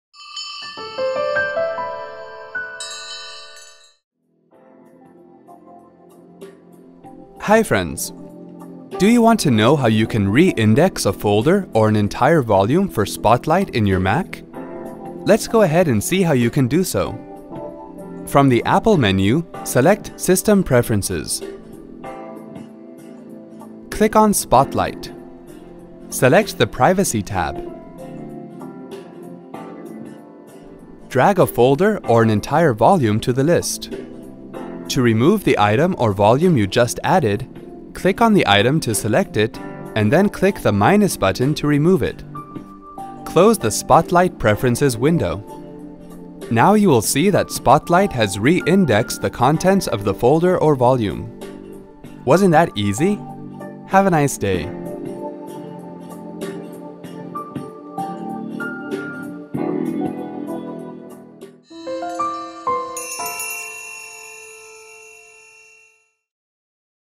Re-index a Folder or an Entire Volume for Spotlight in Mac® OS X™
Re-indexing  Folder and Volumes on Mac OS X: Do you want to know how you can re-index a folder or an entire volume for spotlight in your Mac®? This video will show you simple steps to re-index a folder or an entire volume for Spotlight in Mac® OS X™.